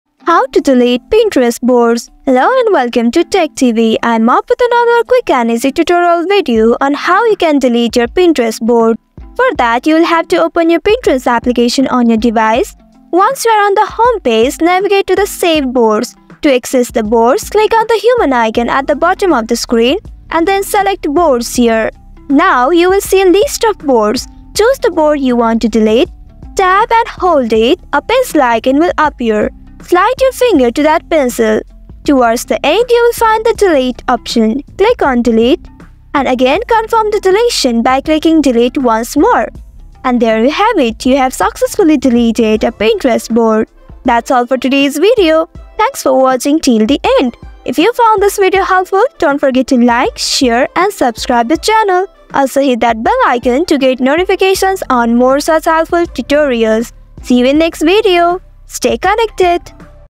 How To Delete Pinterest Boards 2024?
Looking to declutter your Pinterest space or simply want to remove some old boards that no longer fit your interests? You're in the right place! This step-by-step tutorial will show you how to easily delete Pinterest boards from your account, freeing up your profile for new inspirations and ideas. Whether it's a board you've outgrown or a project you're no longer pursuing, learn how to tidy up your Pinterest account in just a few clicks. Watch now to streamline your Pinterest experience and keep your profile fresh and relevant.